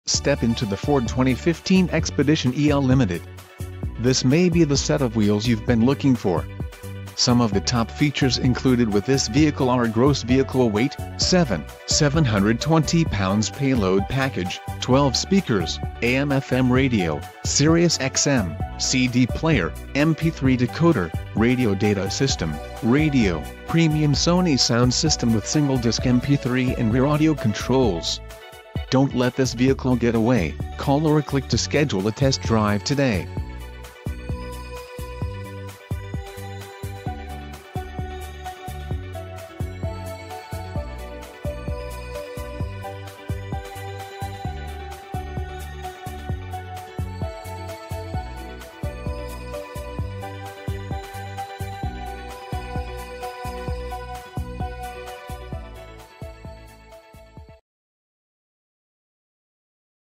2015 Ford Expedition EL Limited
Clean CARFAX. Black 2015 Ford Expedition EL Limited 4WD 6-Speed Automatic EcoBoost 3.5L V6 GTDi DOHC 24V Twin Turbocharged Expedition EL Limited, 4WD, **LOCAL TRADE**, **MOONROOF / SUNROOF**, **NAVIGATION / NAVI / GPS**, **CLEAN CARFAX**.

Awards:
  * 2015 KBB.com Brand Image Awards

Reviews:

  * If you want to tow, haul, or just carry everyone you know plus their friends, the 2015 Ford Expedition may just be the ticket. The new engine should improve fuel economy - relatively speaking - and this big rolling box has excellent cargo space. Source: KBB.com
* Redesigned for 2015 welcome in the all new Ford Expedition. A new aggressive front end creates a bold presence that's perfectly matched to the large and capable Ford SUV.